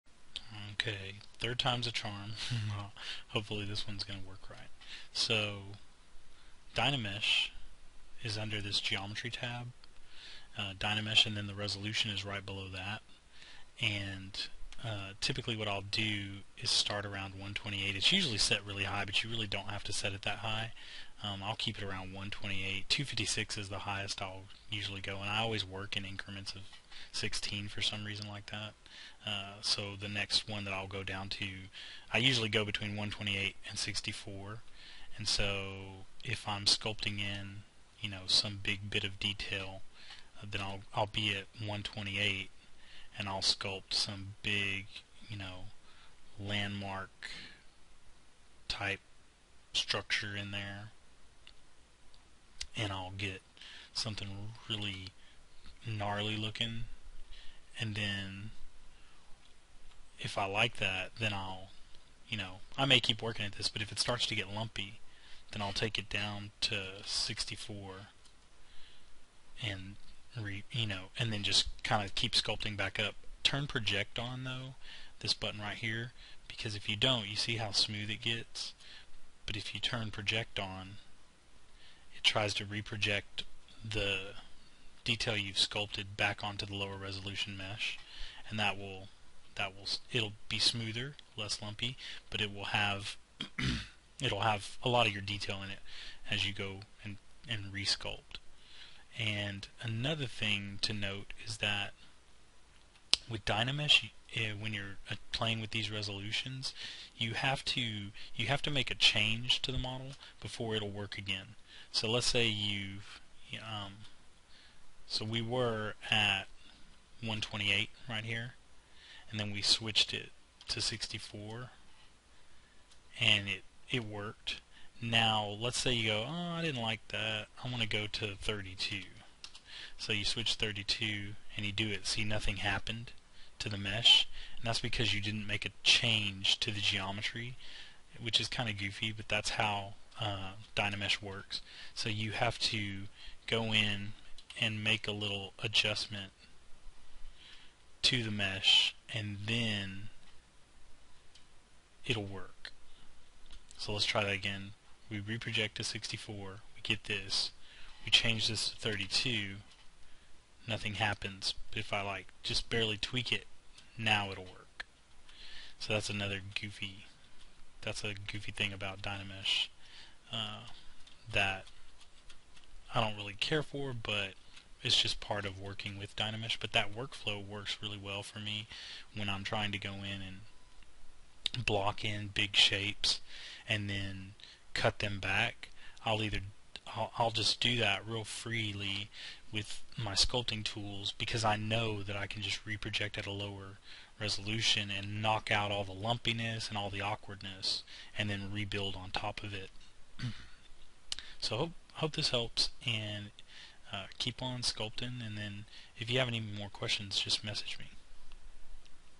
Dynamesh Workflow Demo
This is a quick video showing my workflow with Dynamesh inside Zbrush.